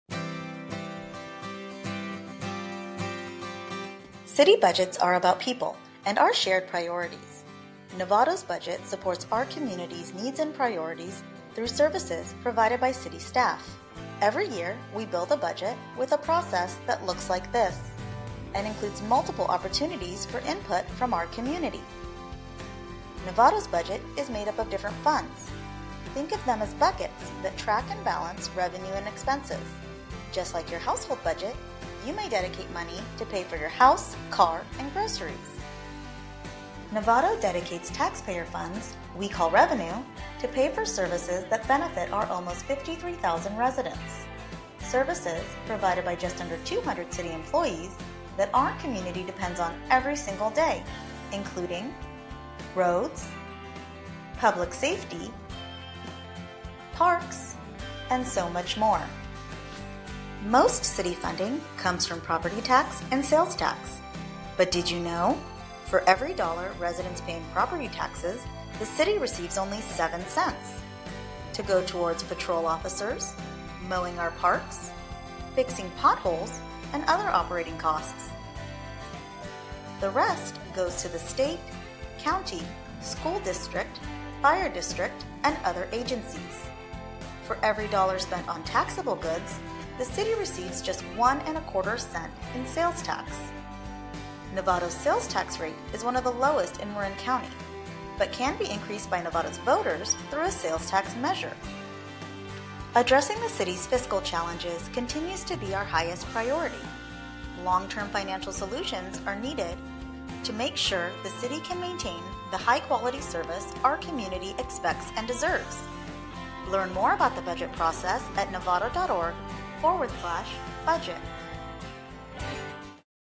Budget 101
Learn about the City of Novato's budget, including how money is allocated, what City services the budget pays for, where your tax dollars go, and how we can address the City's fiscal challenges together.